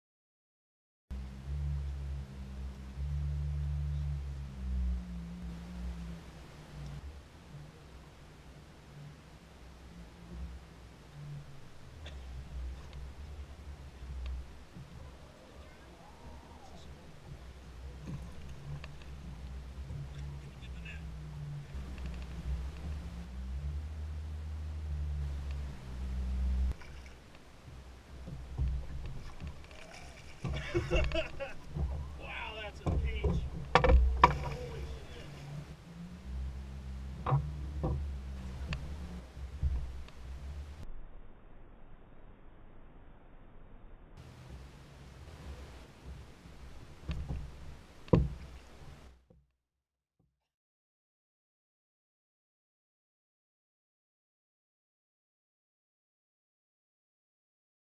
Spoon Smallmouth Bass at the Dot // Wyandotte County Lake KS USA
8/30/15 Caught a nice keeper sized smallmouth bass on a Bink's Spoon white fishing white bass. Wyandotte County Lake, KS